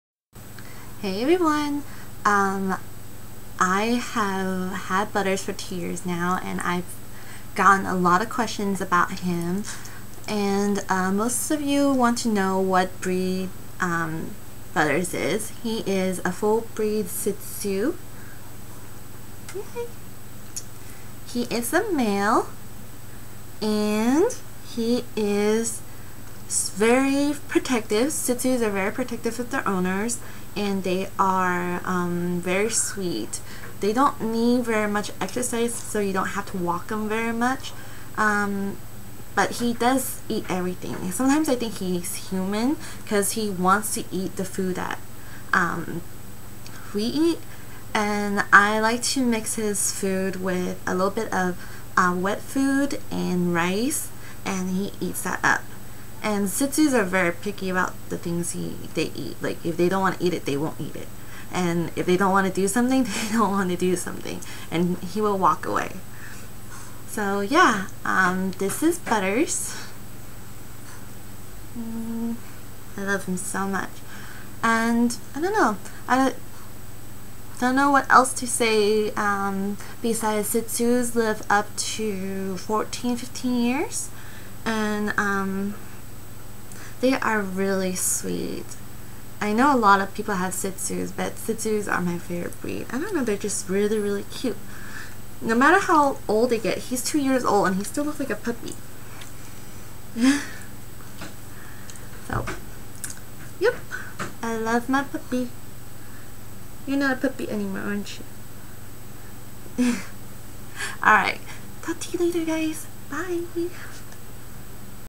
Hello Butters! My Shih Tzu
Hey guys!

You guys seen me post videos & Pictures of Butters all the time and I've gotten a lot of question about him, and here is just a short video explaining how long i've had him and shih tzu's personalities.

They are really one of the sweetest breed of dogs you can own as a pet. He is very protective of me and he will attack anyone who gets near me if I am trying to rest. I love him to death.

He has given me a joy I never thought I could have.